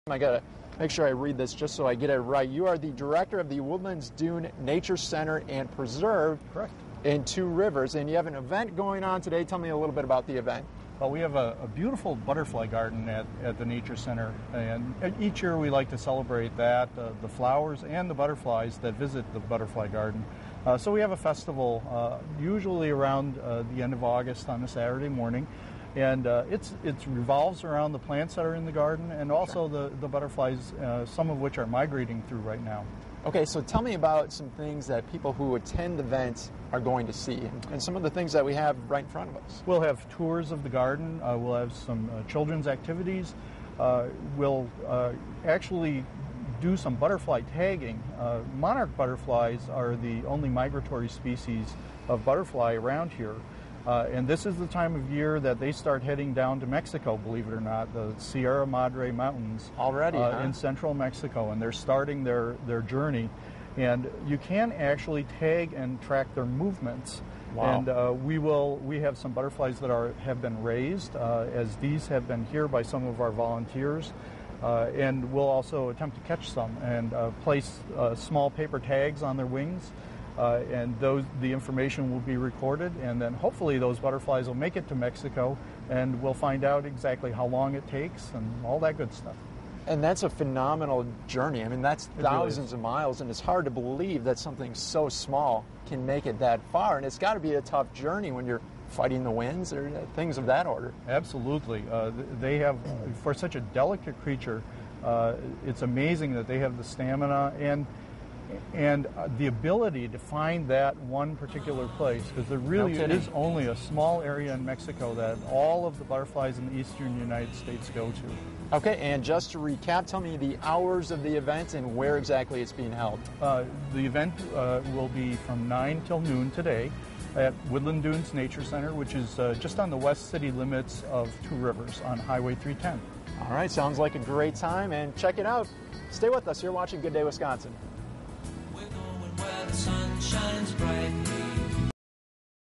Butterfly Garden Festival
Celebrate nature, more specifically butterflies at Woodland Dunes Nature Center in Two Rivers.